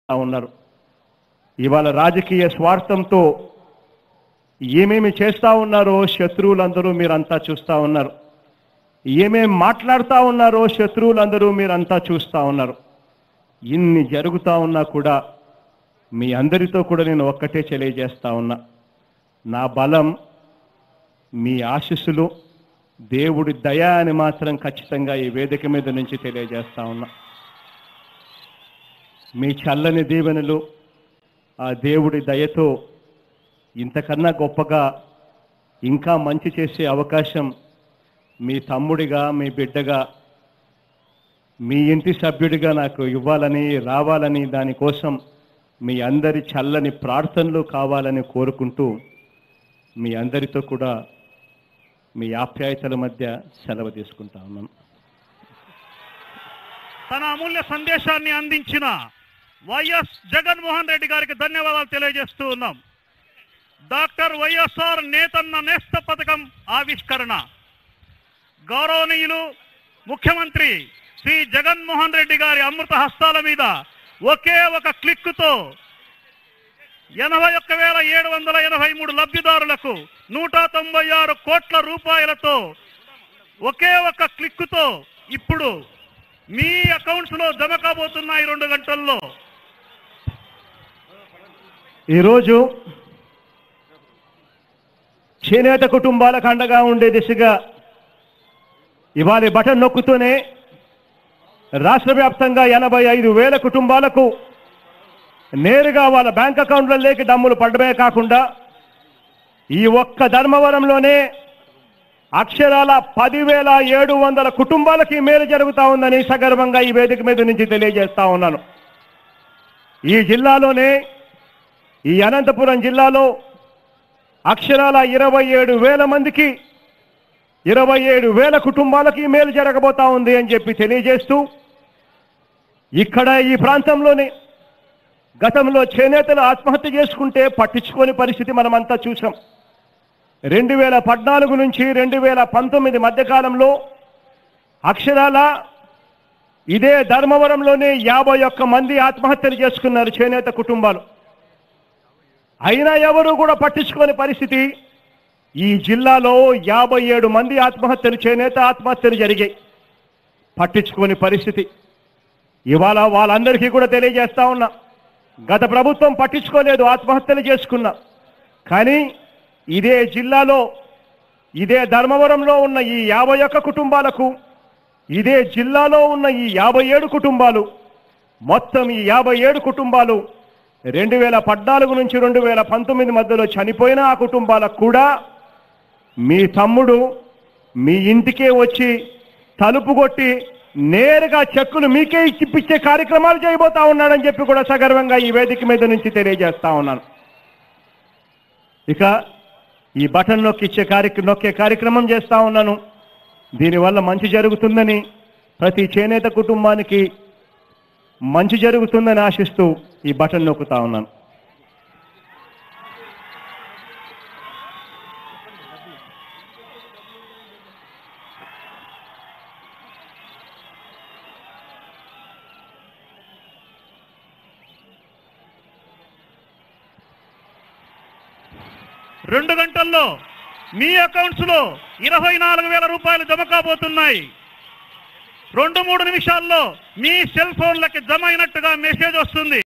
మీ ఆశీస్సులు దేవుని దయ నా బలం | AP CM YS Jagan | YSRCP
Hon'ble CM of AP Participation in Launching YSR NETANNA NESTAM SCHEME at Dharmavaram

Stay Tune into Tulasi News for 24 hour news.